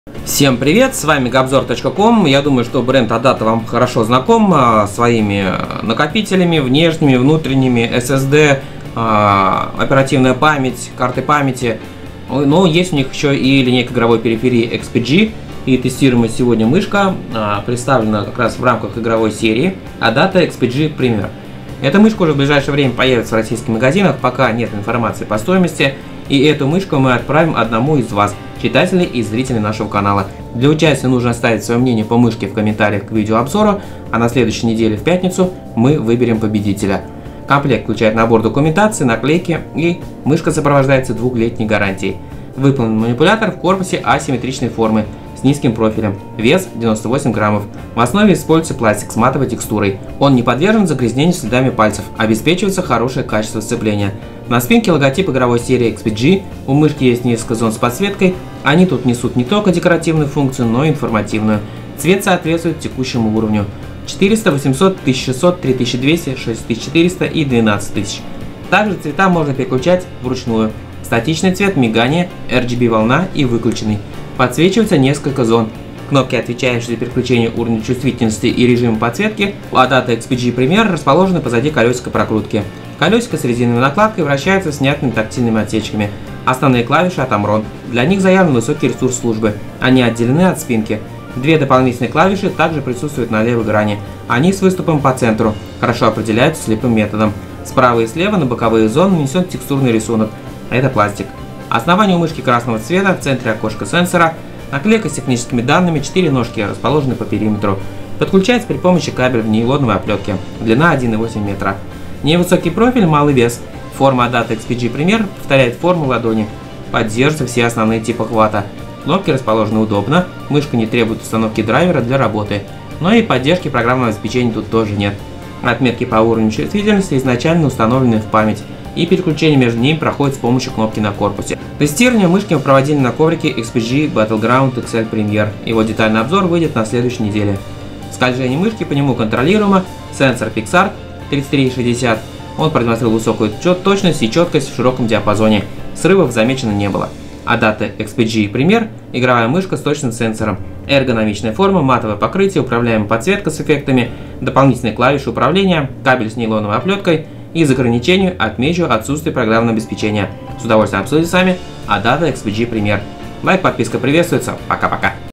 ADATA XPG Primer обзор игровая мышка с точным сенсором
ADATA XPG Primer – игровая мышка с точным сенсором. Эргономичная форма, матовое покрытие, управляемая подсветка с эффектами, дополнительные клавиши управления, кабель с нейлоновой оплеткой. Из ограничений отметим отсутствие программного обеспечения.